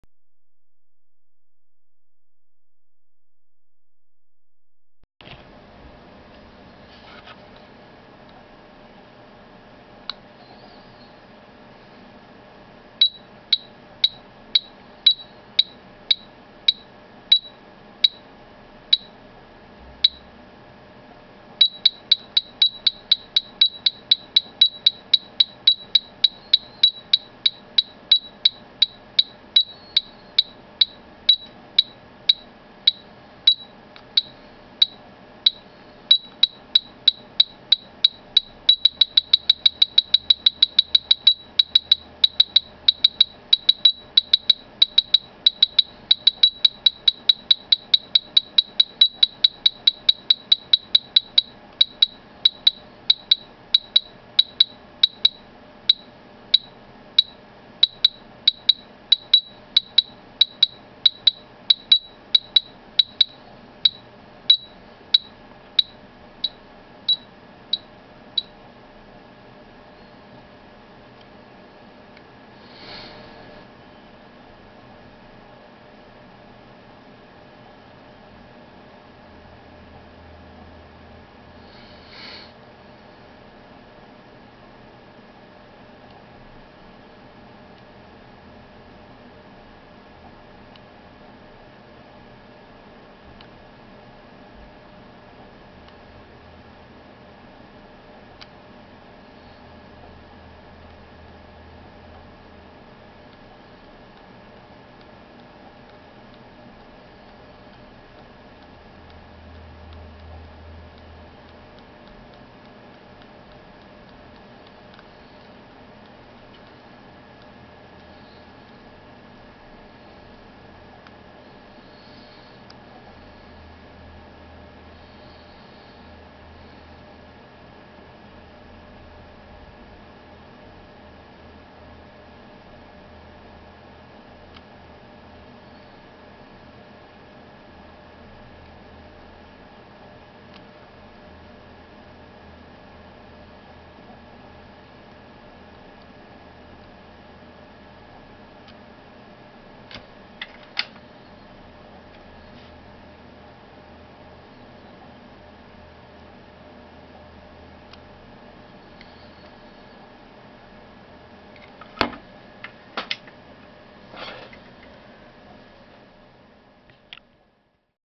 ENO EMT-320
Компактный 3-в-1 девайс ENO на клипсе, включает тюнер, метроном и тон-генератор.

 - 5 режимов: chromatic, guitar, bass, violin, ukulele
 - Неоновая подсветка
 - поворотный экран 360°
 - Маленкий и удобный. Не нужно подключать, просто крепиться на клипсу на перо гитары.
 - авто-выключение при простое. Питание батарейка-таблетка CR2032.
 микроподстройка "ля": 430-450 Гц
 Темп метронома: 30-250 ударов в минуту
 Установка сильной доли на любой счёт
 регулировка громкости
 размер: 45x48x72 мм
 вес: 55гр